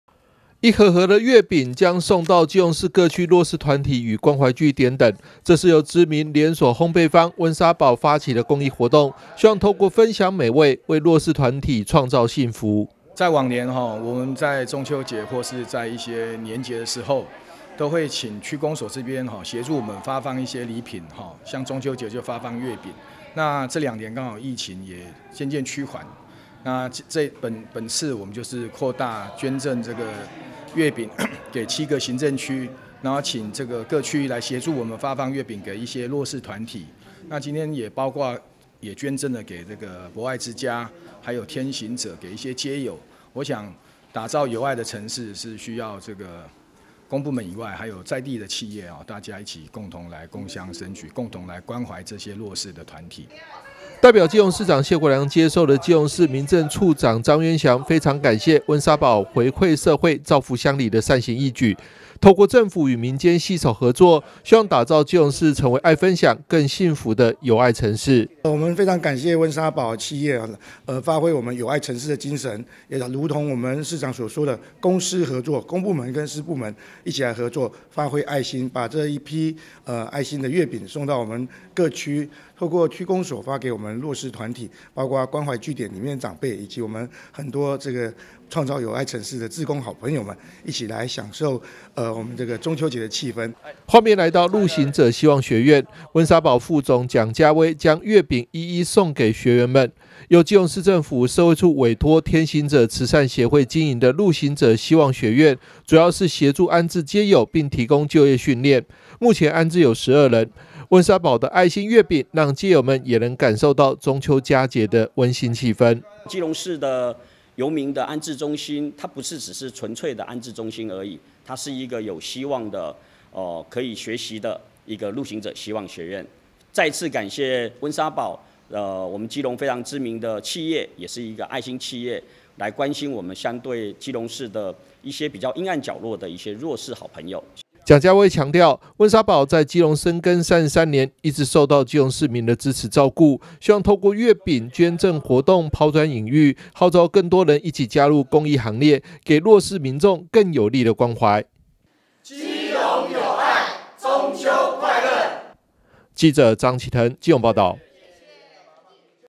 中秋節 公益|基隆 名店|溫莎堡 烘焙坊|愛心 月餅|弱勢戶 街友|2023 中嘉新聞|愛分享更幸福 溫莎堡做公益 兩千盒月餅送基隆弱勢團體
基隆知名烘焙坊溫莎堡捐近兩千盒月餅給弱勢團體 分享美味傳遞幸福

（中嘉新聞記者張啟騰/基隆採訪報導）
迎接中秋節，基隆知名連鎖烘焙坊-溫莎堡特別舉辦公益活動，捐出近兩千盒月餅給基隆市政府與基隆市天行者慈善協會，希望分享美味創造幸福，讓包含街友在內的基隆弱勢民眾也能開心度過中秋佳節，透過公私協力，一起打造基隆成為有愛城市。

一盒盒的月餅將送到基隆市各區弱勢團體與關懷據點等，這是由知名連鎖烘焙坊-溫莎堡發起的公益活動，希望透過分享美味為弱勢團體創造幸福。

代表基隆市長謝國樑接受的基隆市民政處長張淵翔非常感謝溫莎堡回饋社會造福鄉里的善行義舉，透過政府與民間攜手合作，打造基隆市成為愛分享更幸福的有愛城市。

畫面來到路行者希望學院，溫莎堡副總蔣家威將月餅一一送給學員們，由基隆市政府社會處委託天行者慈善協會經營的路行者希望學院主要是協助安置街友並提供就業訓練，目前安置有12人，溫莎堡的愛心月餅讓街友們也能感受到中秋佳節的溫馨氣氛。

蔣家威強調溫莎堡在基隆深耕33年，一直受到基隆市民的支持照顧，希望透過月餅捐贈活動拋磚引玉，號召更多人一起加入公益行列，給弱勢民眾更有力的關懷。